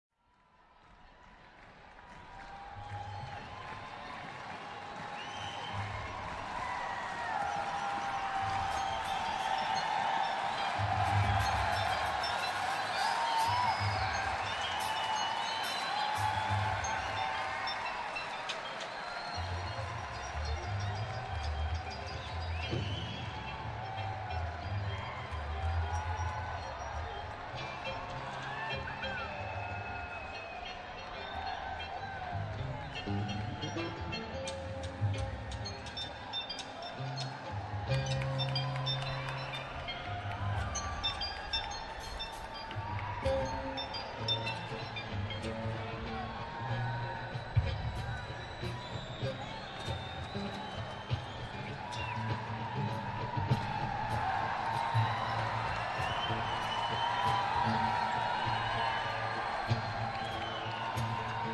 Grateful Dead ~ 08.5 Tuning 2 ~ 03-24-1995 Live at The Coliseum in Charlotte, NC
Grateful Dead live at The Charlotte Coliseum in Charlotte, NC on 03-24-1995.
Song 8.5 of 18. Background made by hotpot.ai/art-generator.
Tuning 2.